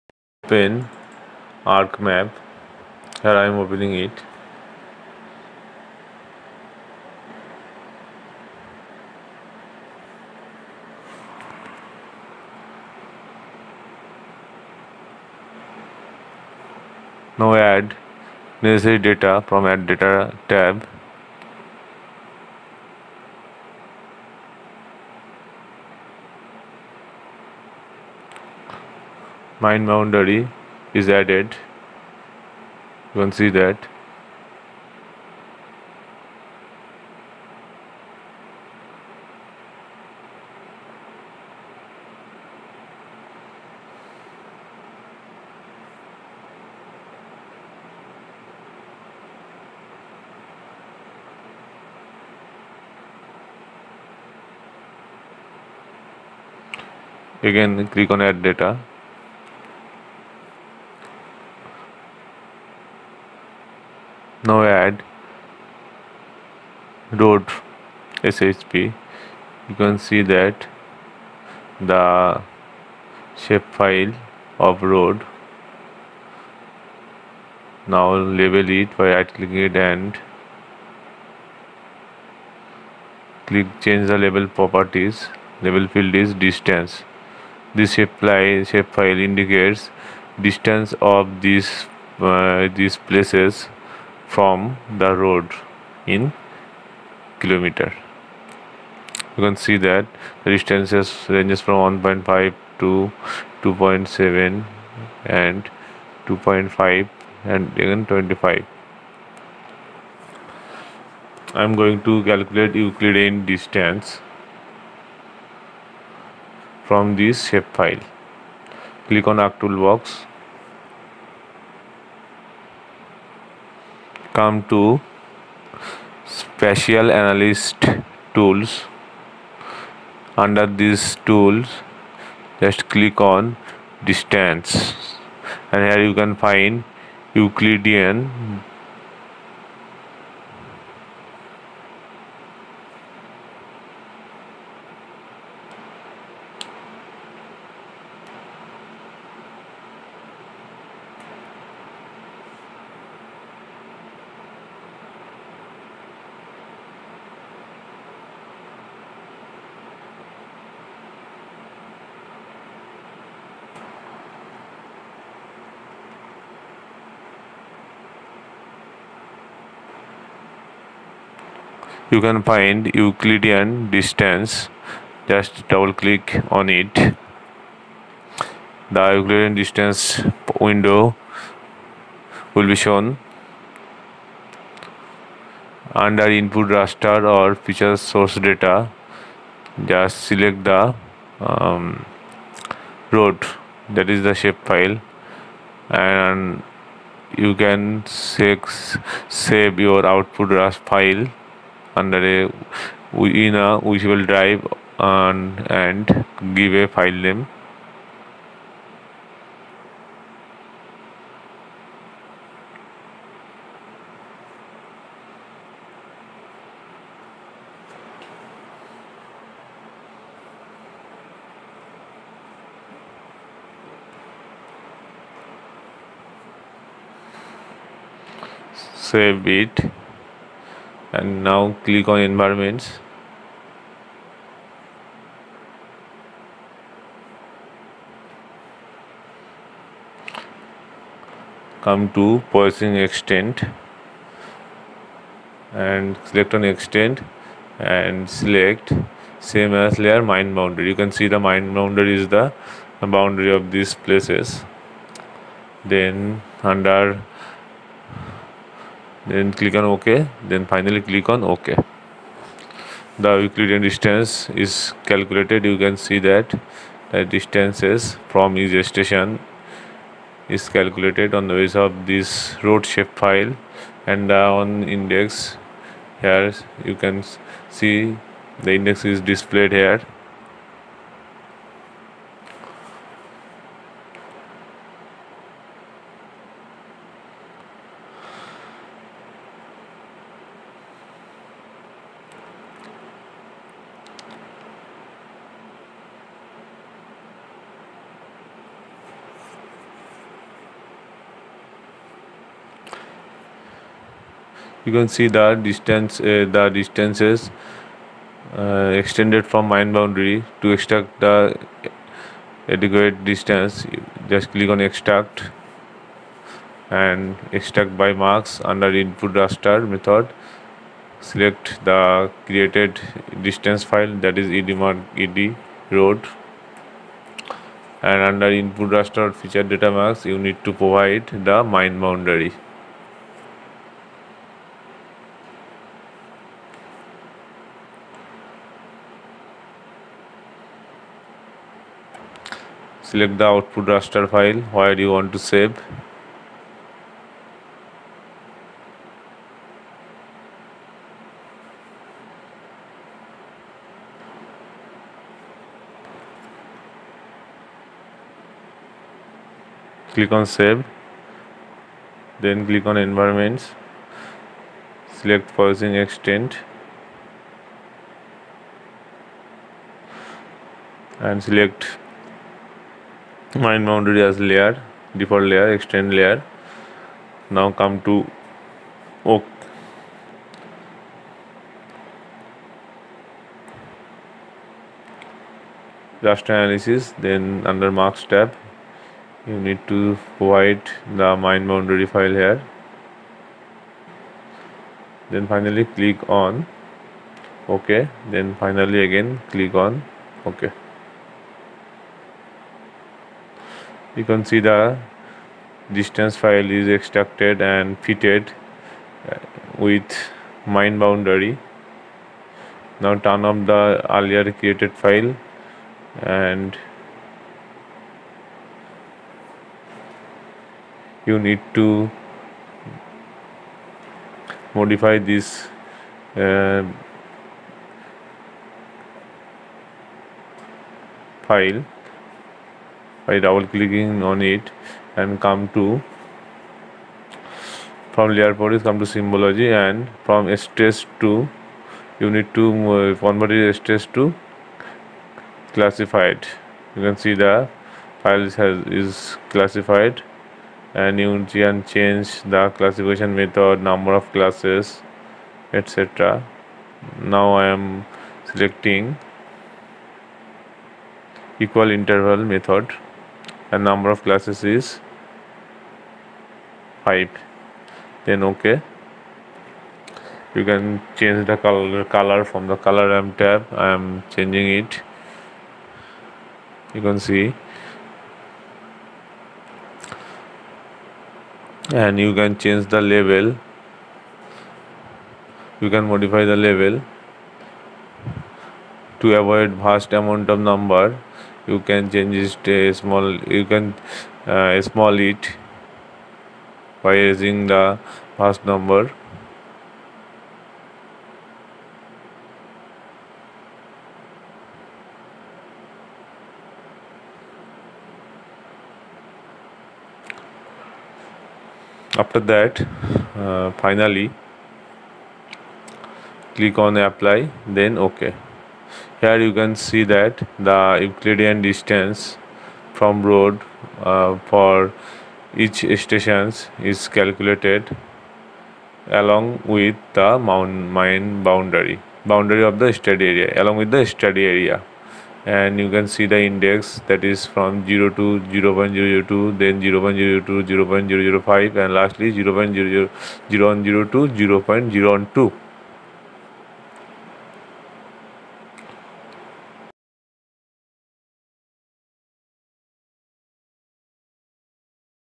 Euclidean Distance in Arc GIS | Buffer Raster & Euclidean Distance with Study Area Extent in Arc Map
Nowadays Arc GIS is widely used to solve various problems. Euclidean Distance is one of them which gives the distance from each cell in the raster to the closest source. This video helps you to get a nice output which covers the full extent of Euclidean Distance computation along with your study area. Alongside Buffer Raster is also computed from spatial analysis tool.